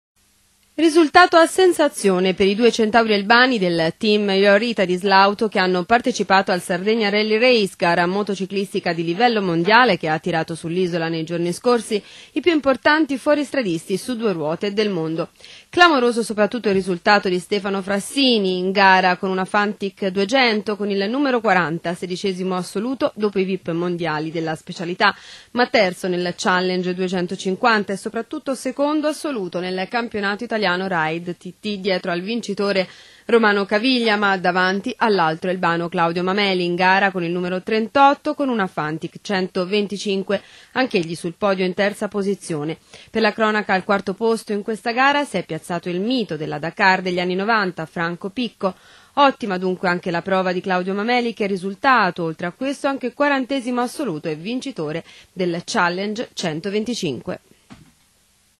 Grande prova degli elbani al "Sardegna Rally Raid"
Stefano Frassini 16.mo assoluto e secondo nel Campionato Italiano Raid TT davanti a Claudio Mameli, alle loro spalle il "mito" Franco Picco